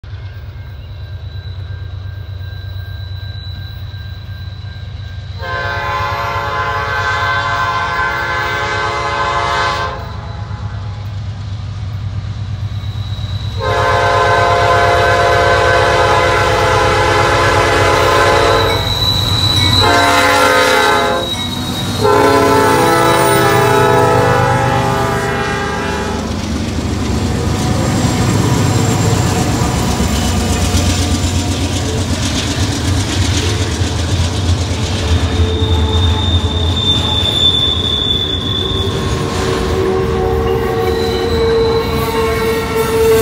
*Iconic GE Sound* NS AC44 Leader With Nathan P5 + ES44AC, WHITEFACE Dash 9, and Freshly Rebuilt AC44
Love these rumbly GEs! That White Face Dash 9 Tho!